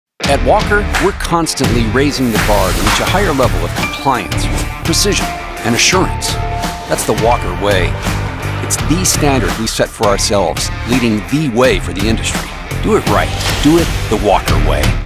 Walker Exhaust | The Walker Way :15
For over 100 years, Walker has set the standard for mufflers, catalytic converters and exhaust components. We call it The Walker Way. From our unparalleled EPA and CARB compliance to our product portfolio boasting OE-style design and fit, Walker strives to instill a higher level of excellence in our parts and people. to find quality exhaust parts that deliver OE-style fit, sound and performance for every application. Do it right. Do it The Walker Way.